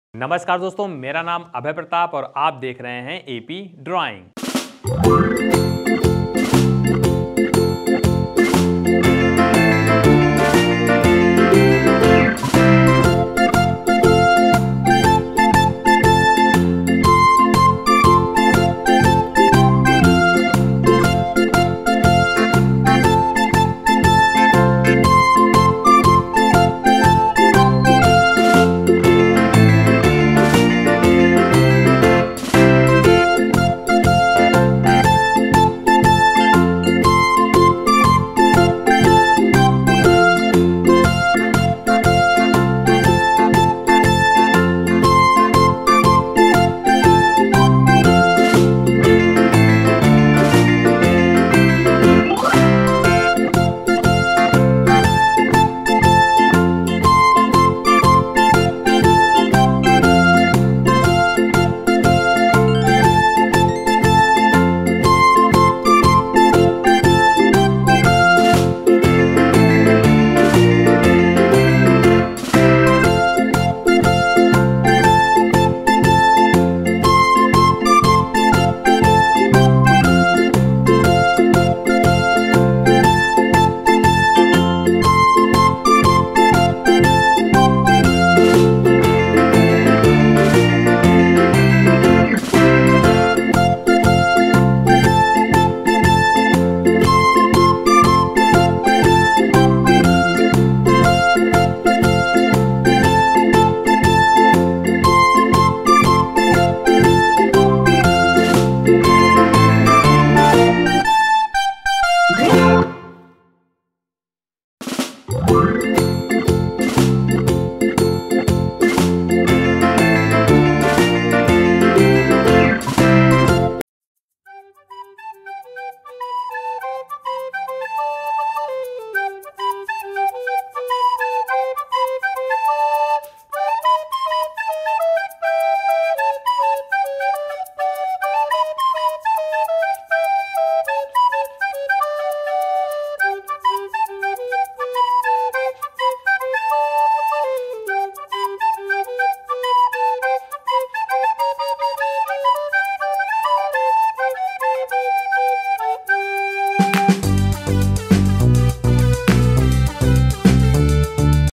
99% लोग गुलाब बनाने का तरीका नहीं जानते | Most Easy Way to Draw Rose | Step by Step Easy Rose
today I am show how to Draw a rose Flower step by step for beginners

Hello Friends I am Abhay Paratap